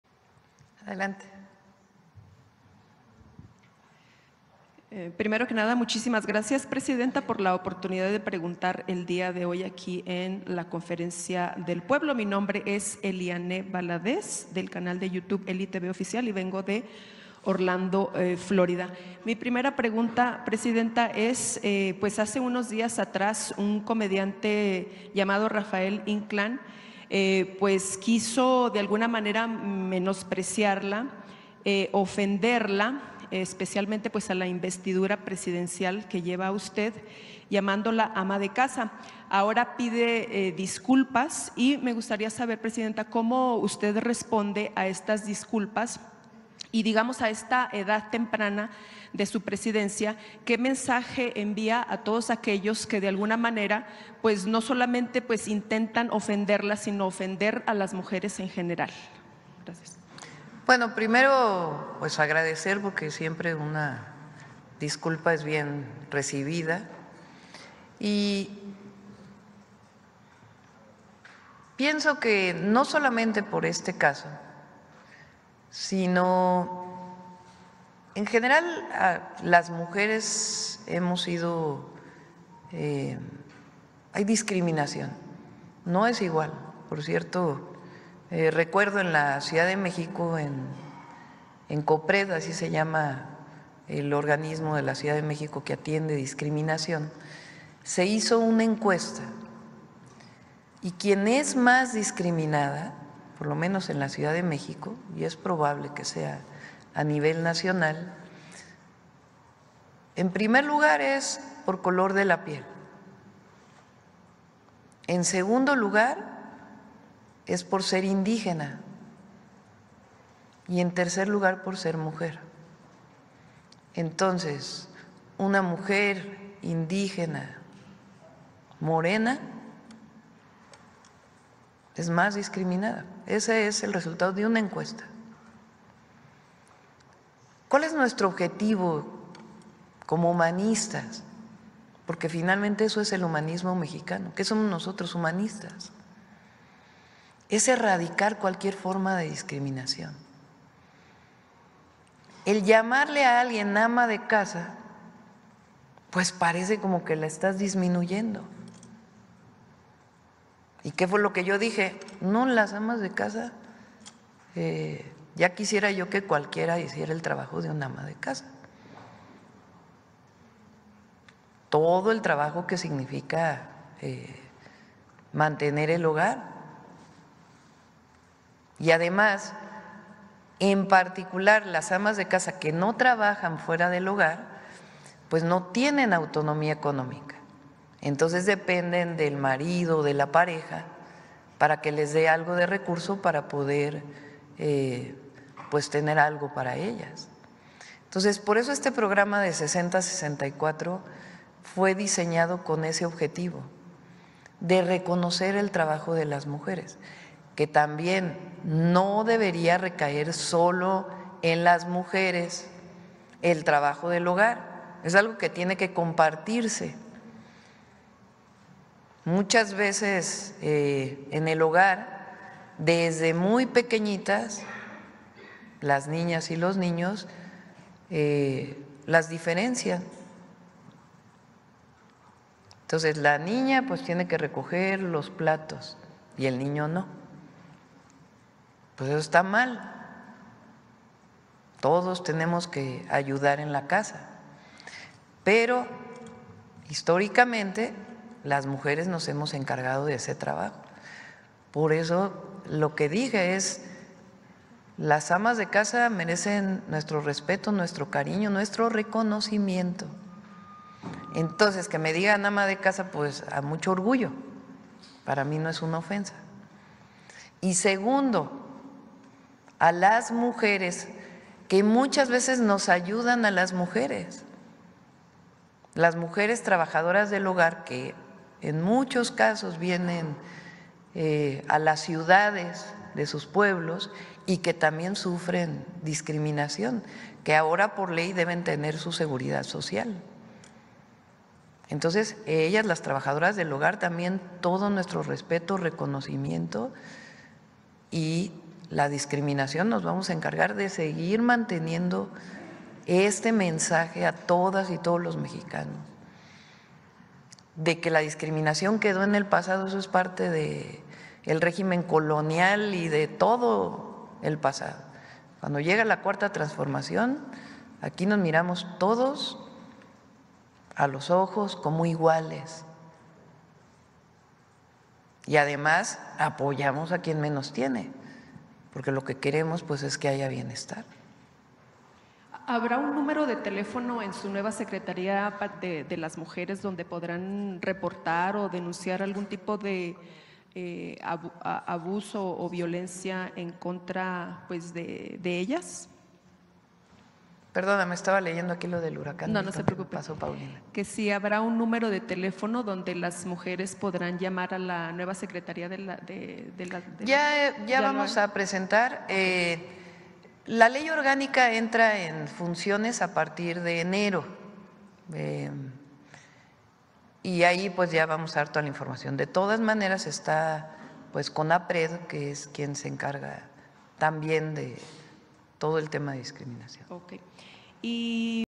Sheinbaum agradece disculpas de infame RafaelInclan por referirse a ella como “ama de casa”
EliTV le pidió a claudiasheinbaum su opinión sobre las disculpas ofrecidas por el comediante RafaelInclan por llamarla ama de casa de manera pεy0r4t1v4, la presidenta agradeció el hecho porque “siempre una disculpa es bien recibida” no solo por este caso, sino porque en general las mujeres han sido d1scr1m1n4d4s. A continuación, recordó cuando la Copred en la CdMx realizó una encuesta que arrojó que una mujer de color e indígena es triplemente d1scr1m1n4d4, por lo que su objetivo es erradicar cualquier forma de d1scr1m1n4c1ón